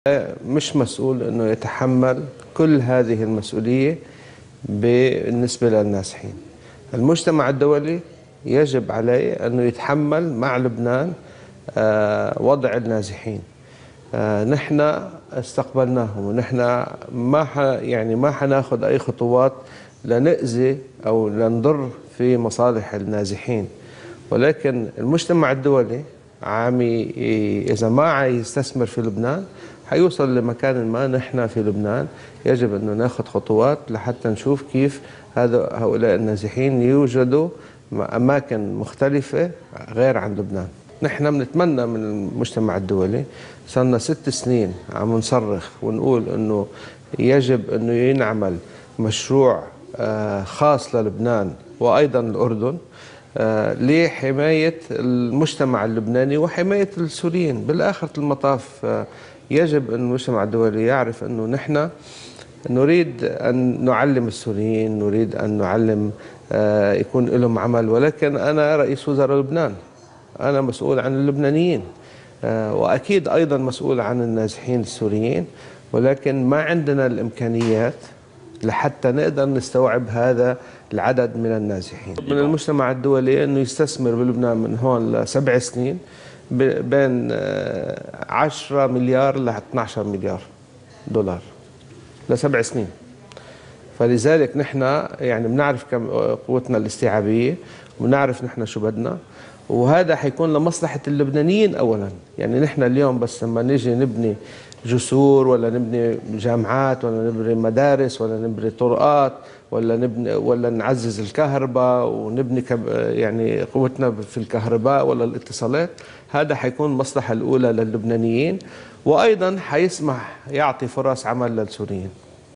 الحريري بجولة أوروبية: نطلب مساعدتنا في أعباء أزمة النزوح
يبدأ رئيس مجلس الوزراء سعد الحريري جولة أوروبية يستهلها غداً الاثنين، من العاصمة الفرنسية
باريس .